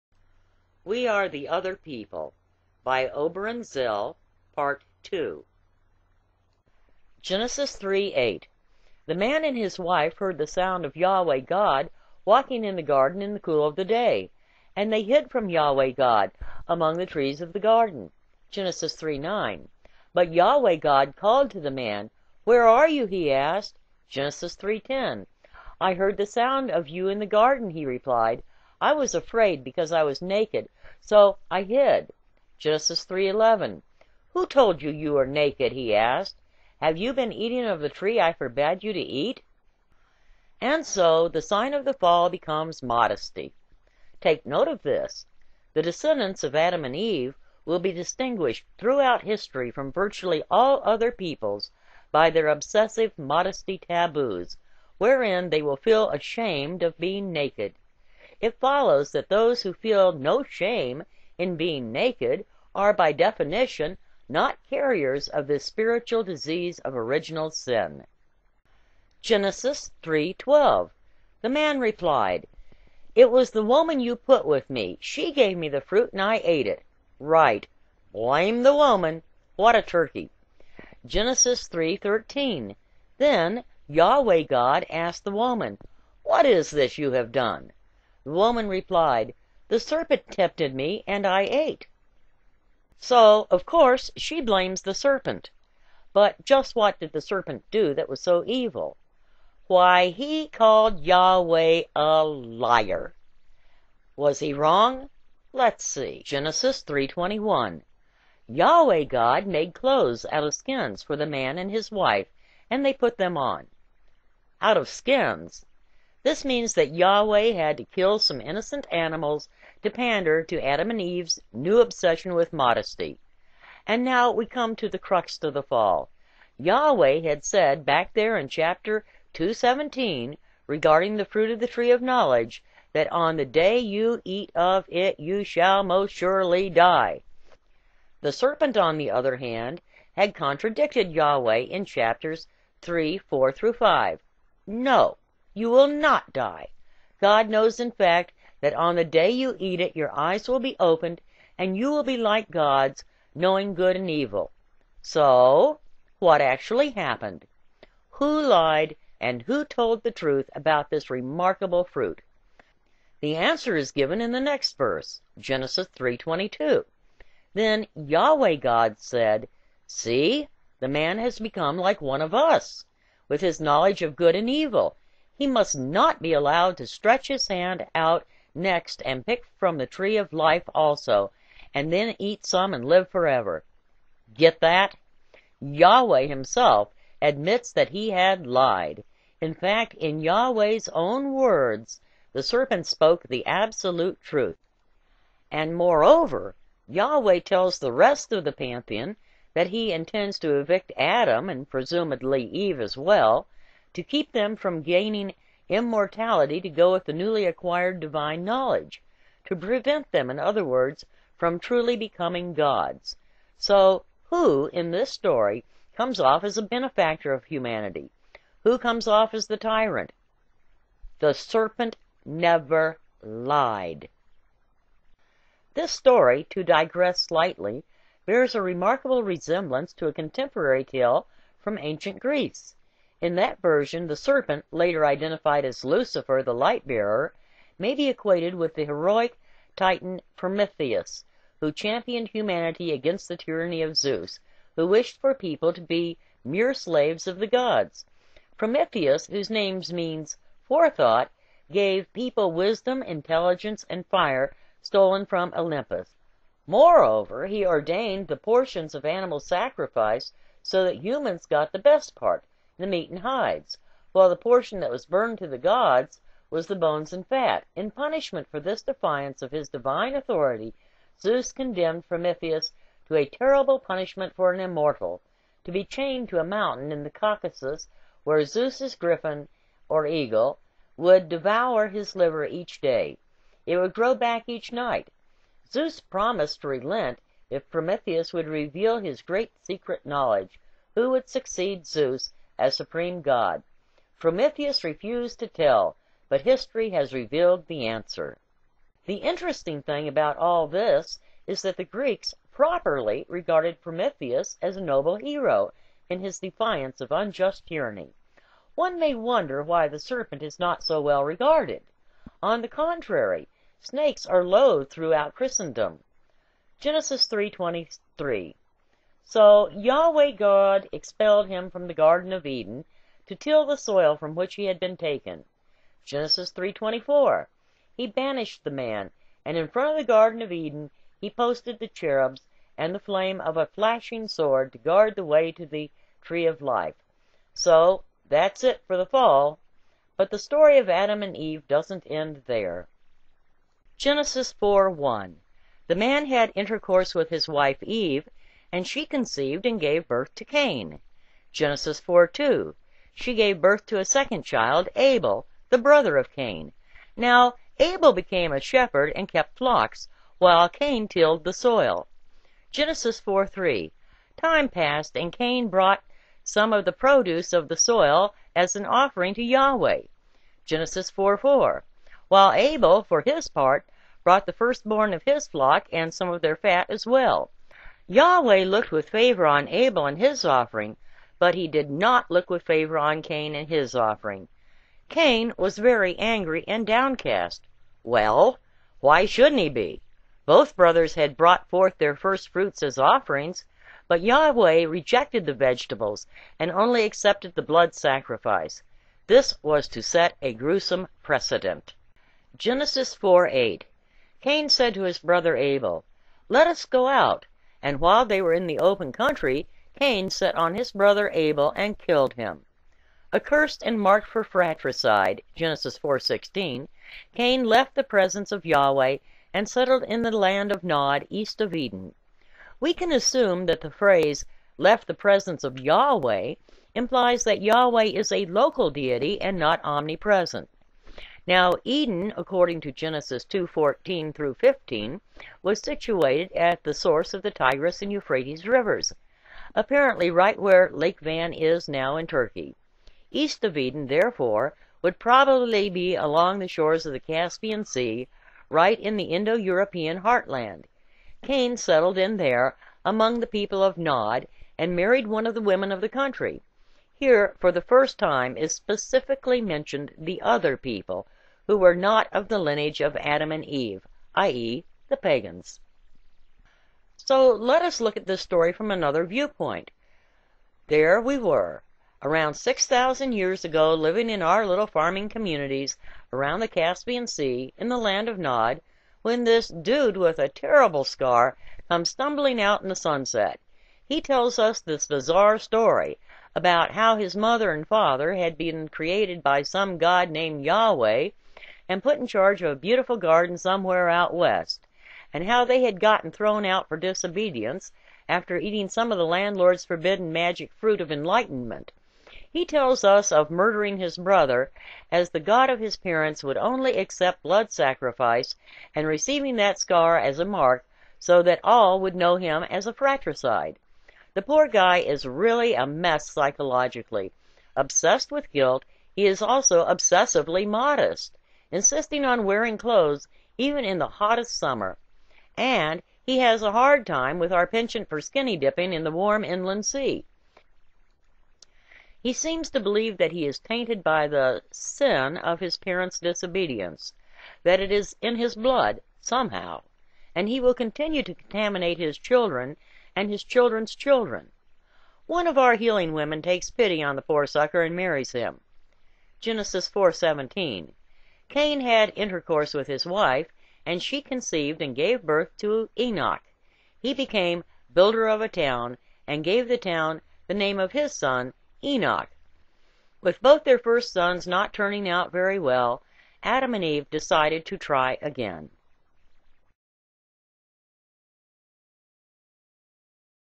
We Are the Other People - Part 2
"We Are the Other People"
by Oberon Zell
Read by SSanf
From  the "Church of all Worlds"